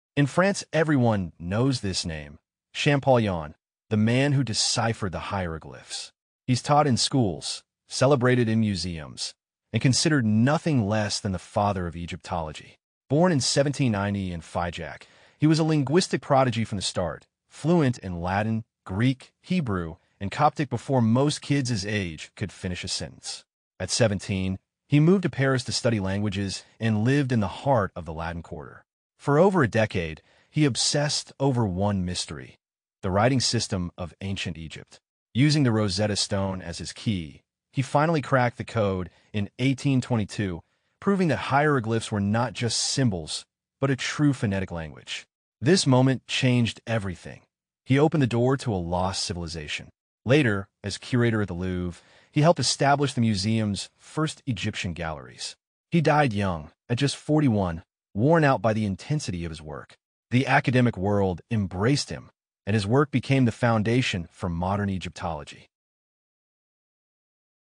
CHAMPOLLION: Cracking the Code of Ancient Egypt
Part of the Legendary Parisians series, this short video tells the story of Jean-François Champollion, the Paris-based linguist who deciphered the hieroglyphs.
Working for years at 28 rue Mazarine, he cracked the Rosetta Stone in 1822—unlocking the language of ancient Egypt and founding a new science: Egyptology.
The academic world embraced him, and his work became the foundation for modern Egyptology.